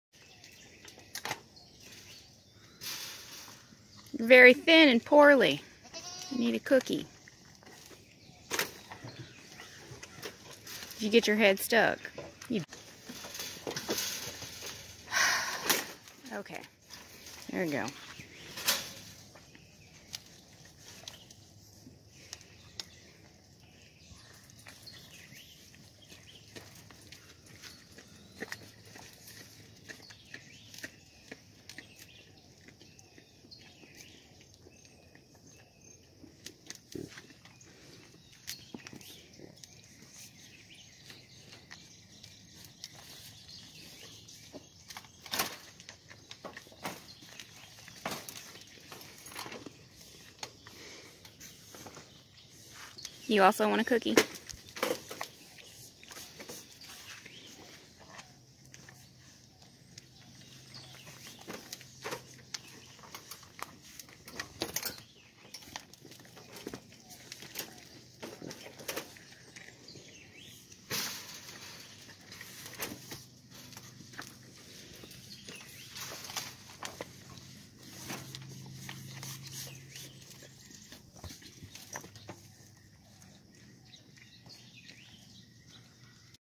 Fattening Up Nibbles
The last of the mama goats to be separated from their kids deserve some carbs. August 2022.